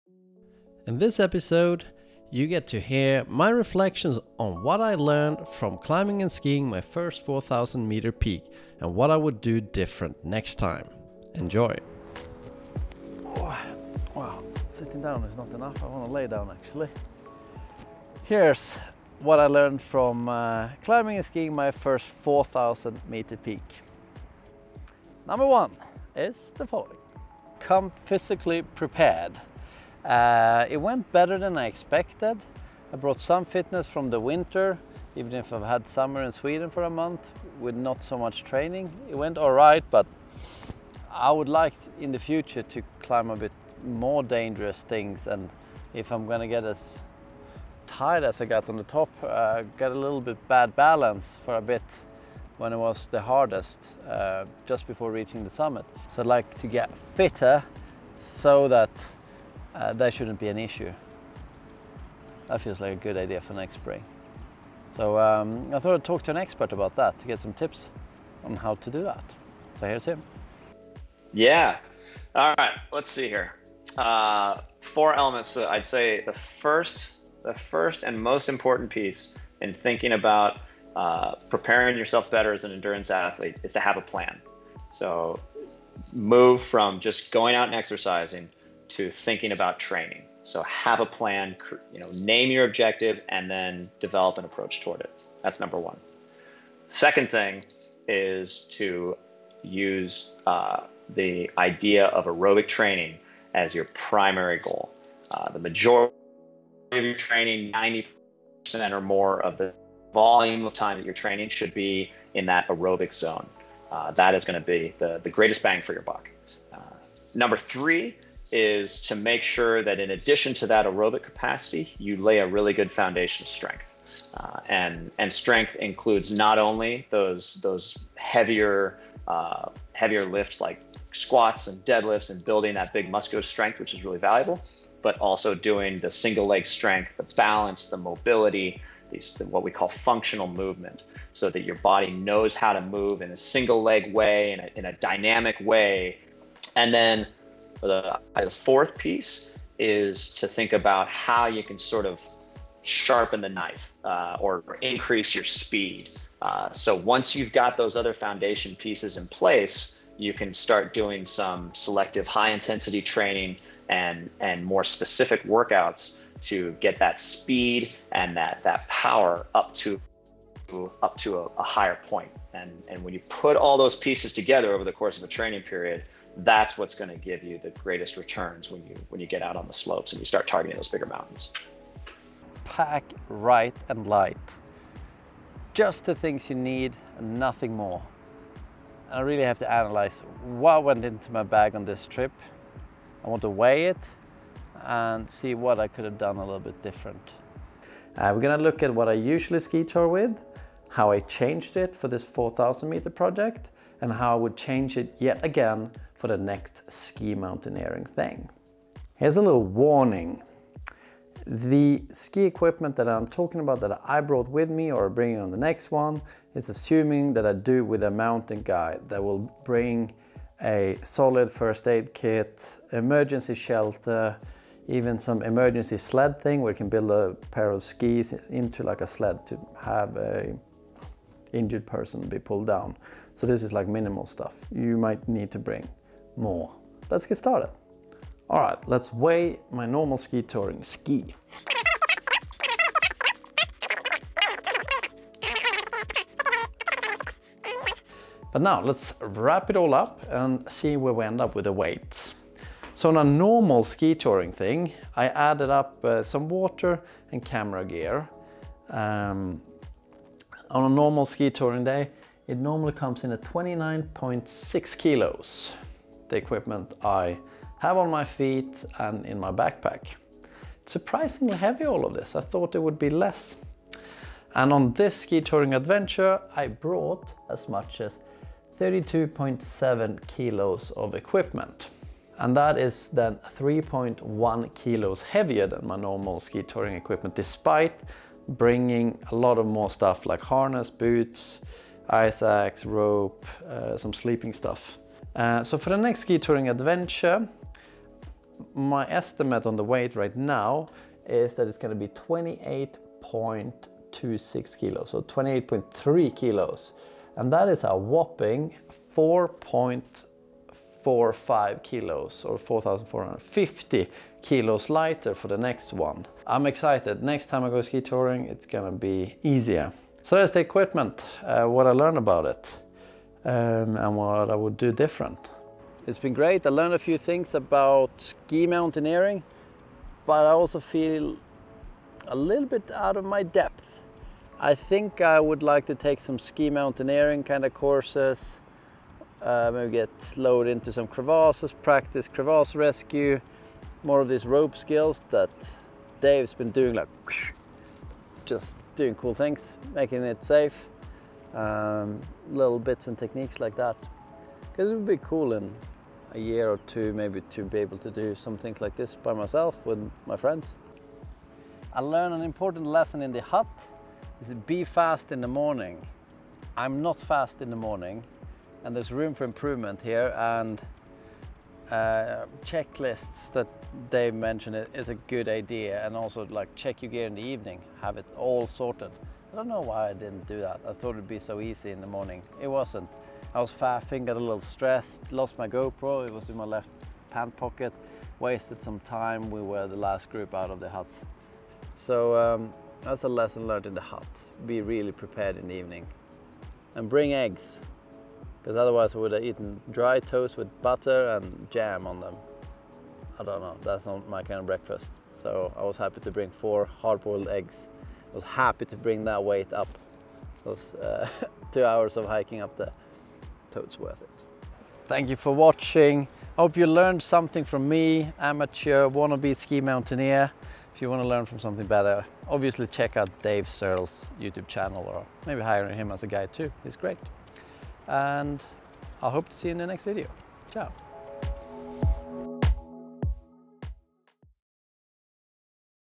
How to Ski Mountaineer | 5 Lessons Learned
In this video, I reflect on what I learned from climbing and skiing the Gran Paradiso, my first 4000m peak. A few topics we cover is how to train for ski touring / mountaineering, what gear I used and will use next time and much more.

Intro: 00:00
00:25 #1 Come physically prepared
01:11 How to train for ski mountaineering
03:02 #2 Pack Right and Light
05:27 #3 Exciting to have soo much to learn
06:02 #4 Be prepared in the hut
06:35 #5 Bring Eggs!!!

Stomp It Camps - Ski camps for adults
Ski Technique | Freestyle | Freeride